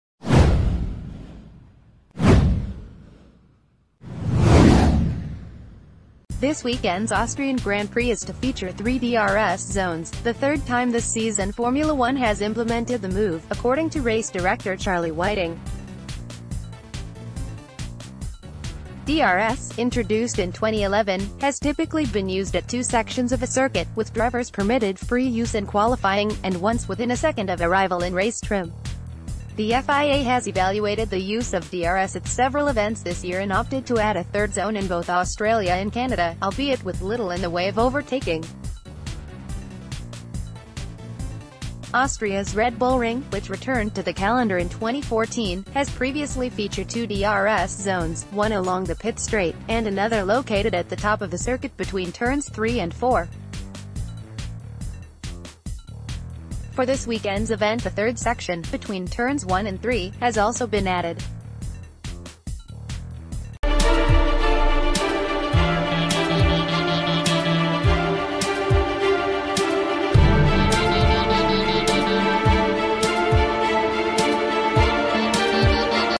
Formula 1: FIA adds third DRS zone for Austrian GP
This weekend’s Austrian Grand Prix is to feature three DRS zones, the third time this season Formula 1 has implemented the move, according to Race Director Charlie Whiting. DRS, introduced in 2011, has typically been used at two sections of a circuit, with drivers permitted free use in qualifying, and once within a second of a rival in race trim. The FIA has evaluated the use of DRS at several events this year and opted to add a third zone in both Australia and Canada – albeit with little in the...